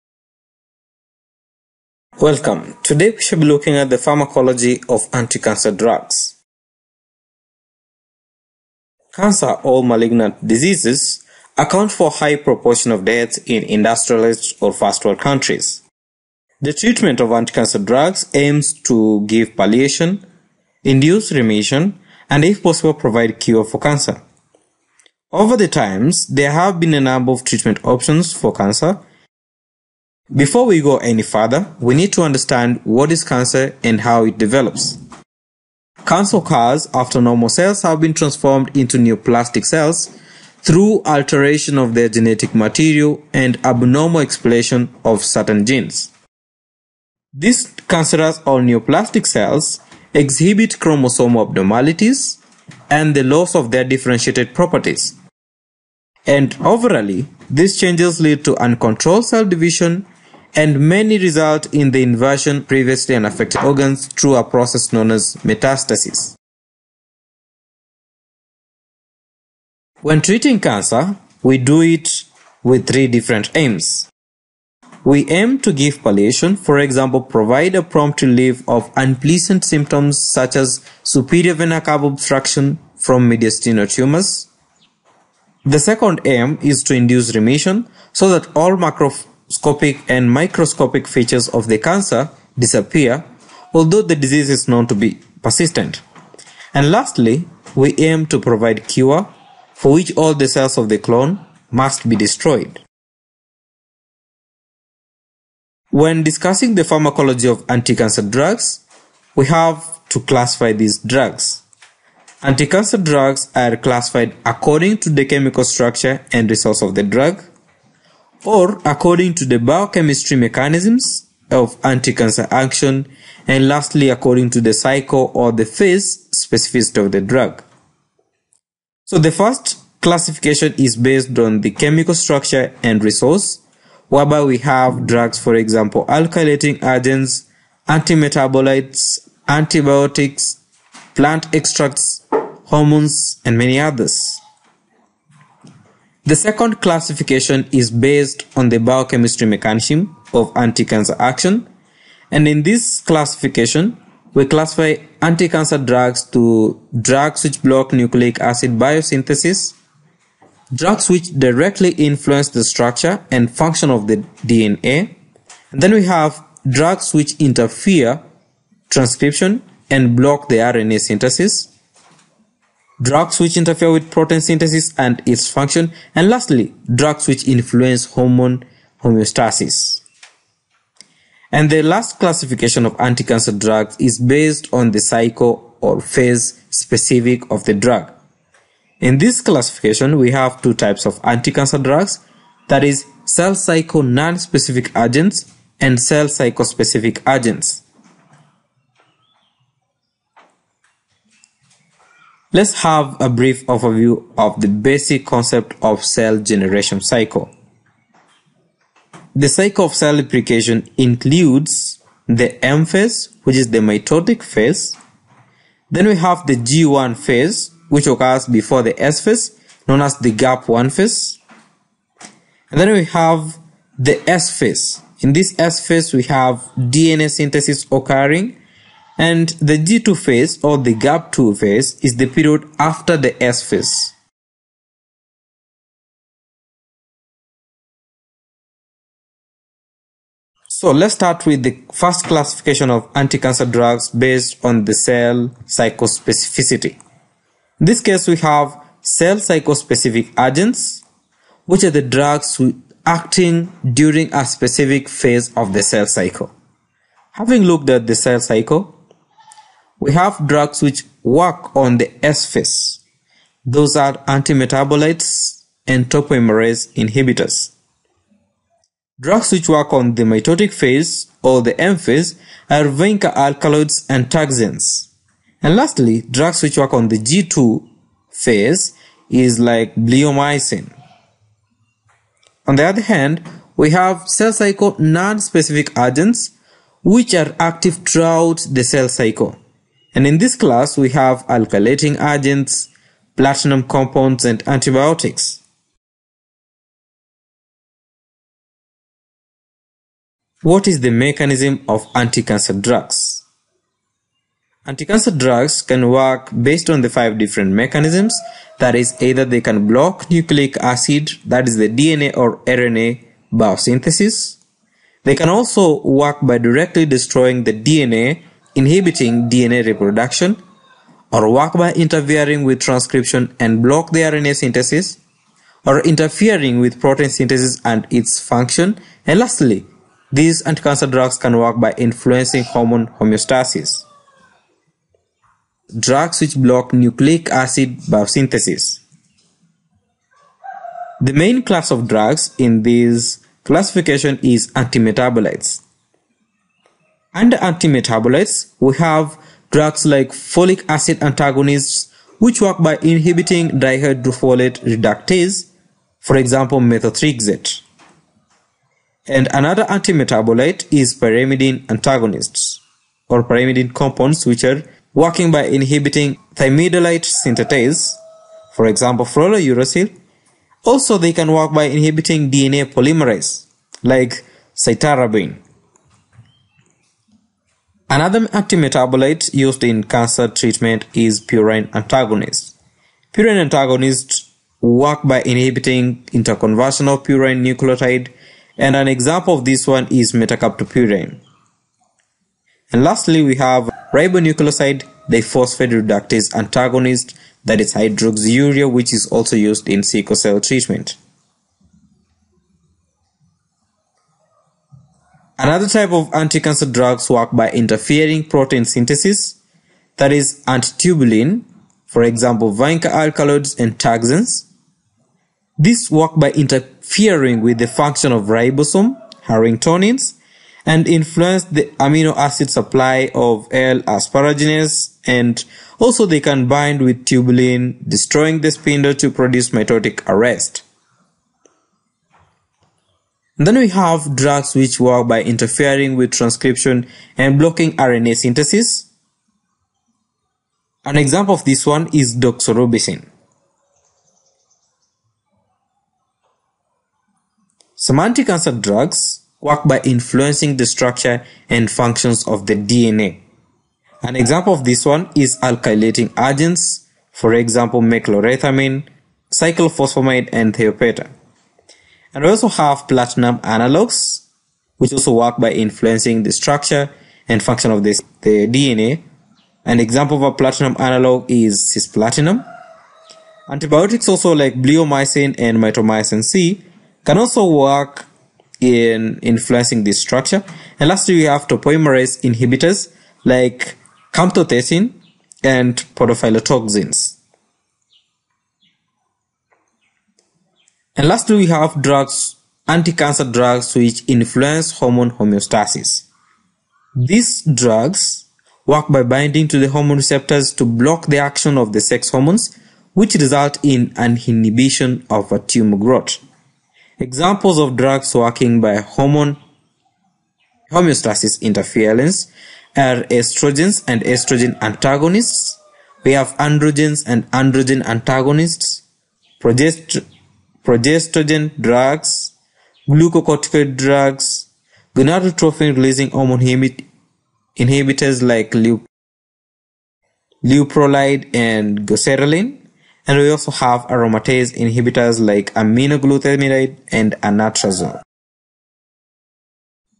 Anticancer Drugs Pharmacology Simplified
The treatment of a patient with cancer may aim to:
Give palliation, for example, prompt relief of unpleasant symptoms such as superior vena cava obstruction from a mediastinal tumour
Induce ‘remission’ so that all macroscopic and microscopic features of the cancer disappear, though the disease is known to persist
Cure, for which all the cells of the clone must be destroyed.
According to the chemical structure and resources of the drug;
According to biochemistry mechanisms of anticancer action;
According to the cycle or phase specificity of the drug